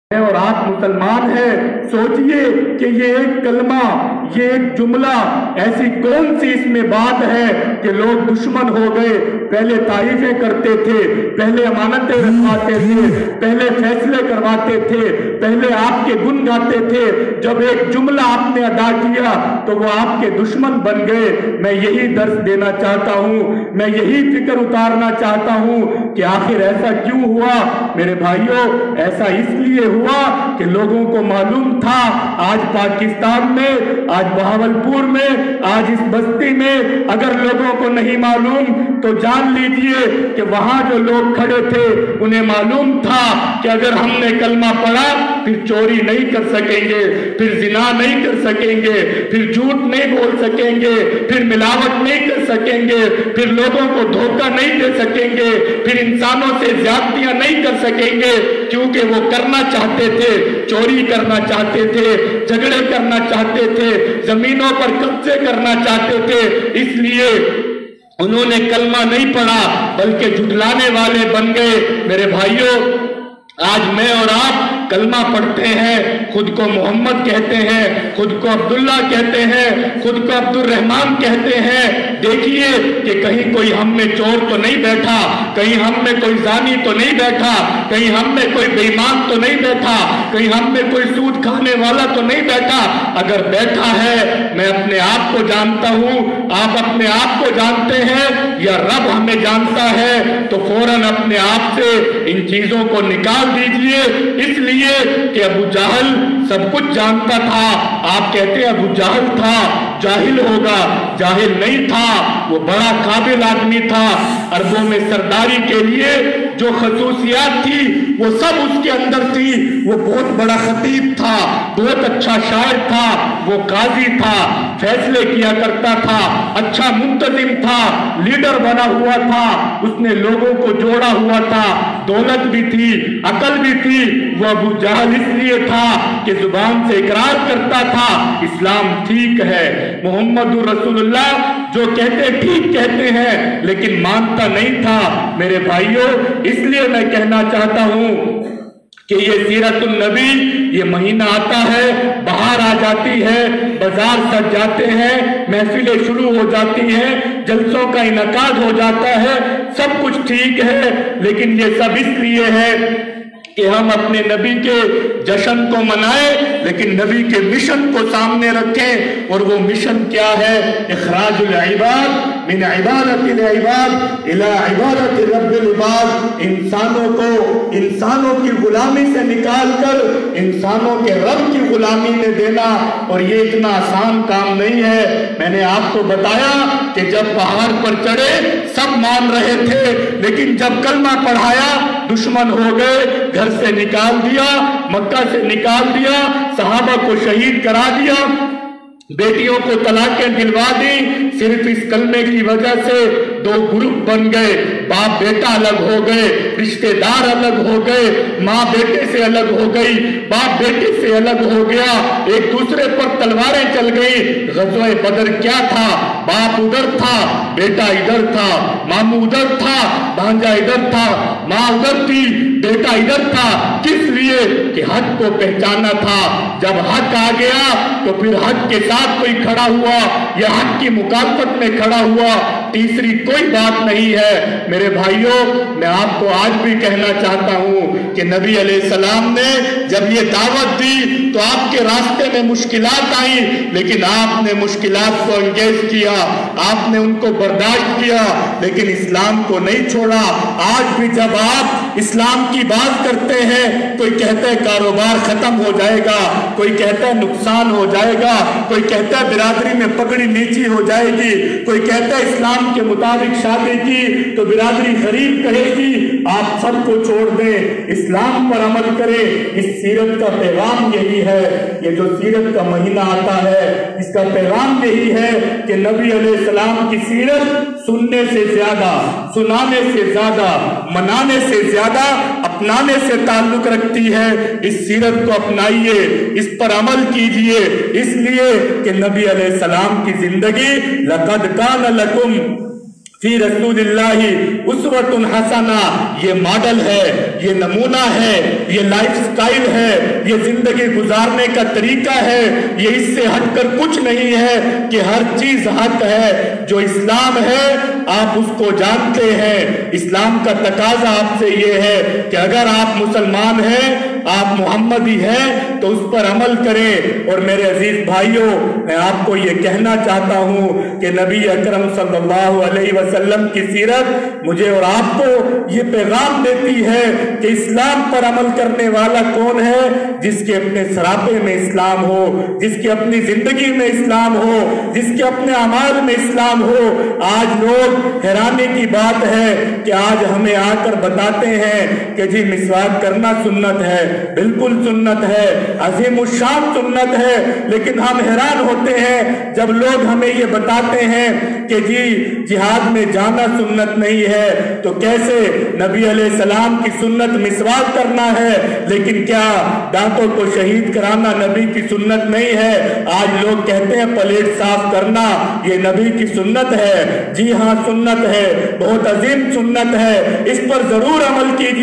سیرت_النبیؐ_کانفرنس خطاب مولانا محمود بشیر
مولانا محمود بشیر
نائب قیم جماعت اسلامی جنوبی پنجاب
سیرت_النبیؐ_کانفرنس
مسجدعائشہ حمیراٹبہ بدرشیر بہاولپور